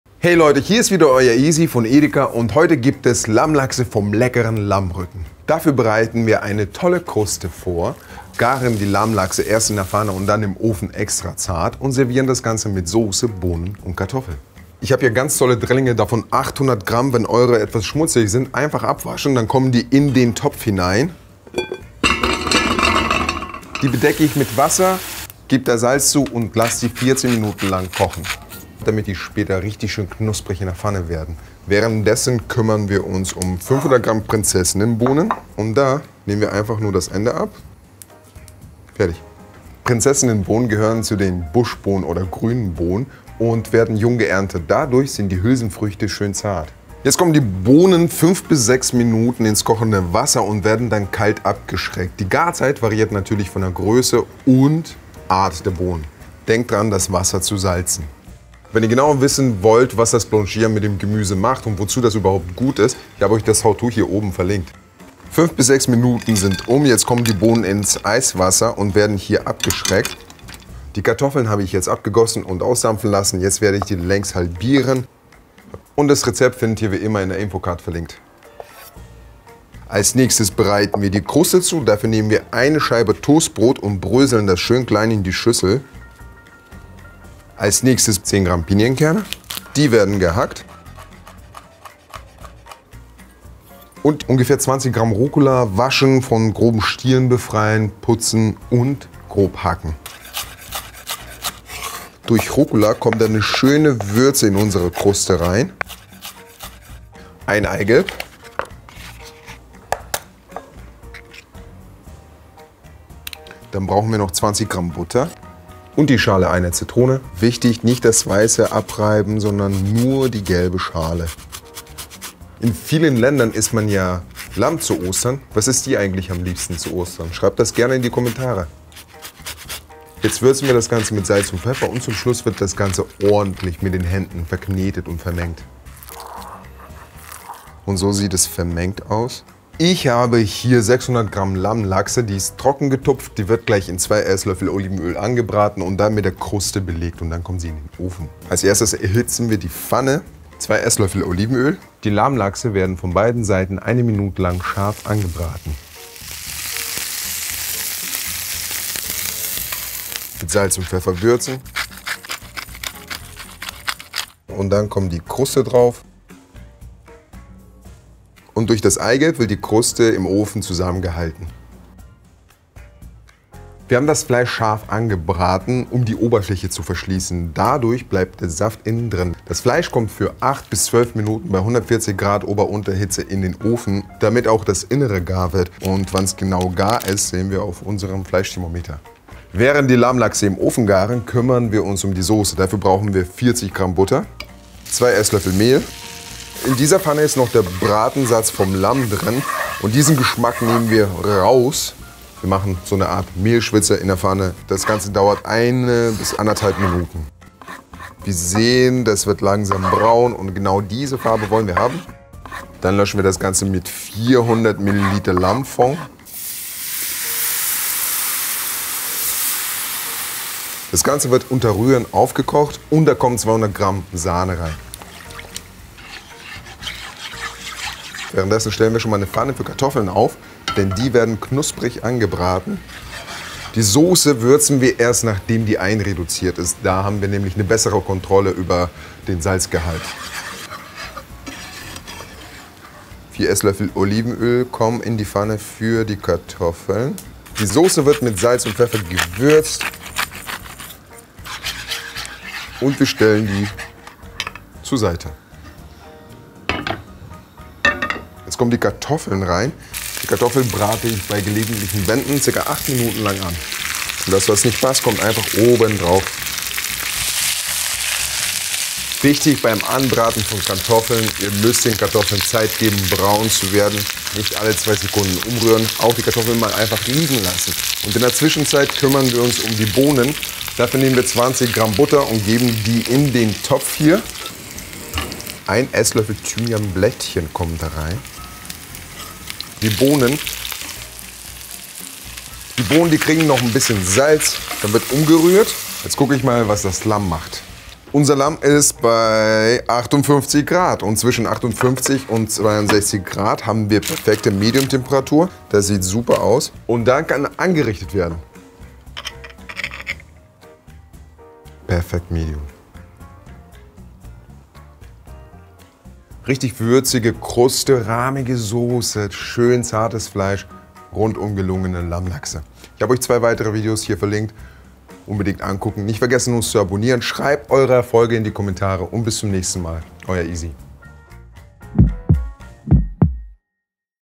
Lammlachse | Zartes Lamm Rezept aus Lammkoteletts zubereiten
► Lammlachse ✔ Zartes Lamm-Rezept aus Lammkoteletts zubereiten ✔ ► Der offizielle Edeka YouTube Kanal – Schnelle und leckere Rezepte für einen unvergesslichen Gaumenschmaus!

Würzig, saftig & zart: Bei unserem Lammlachse-Rezept werden die Lammkoteletts optimal in der Pfanne und im Backofen zubereitet. Die Fleisch-Genießer:innen wissen es - Lammrückensteak liefert die perfekte Voraussetzung für herrlich zartes Lammfilet, wobei unsere leckere Marinade für die perfekte Würze sorgt. Kombiniert mit Kartoffeln und Bohnen, kocht ihr euch mit diesem Lamm-Rezept zu einem besonderen Glücksmoment.

► Ihr wollt die Lammlachse selber machen? Hier geht’s zum Rezept

► Lust auf noch mehr leckere Rezepte? Dann meldet euch jetzt für den EDEKA Newsletter an: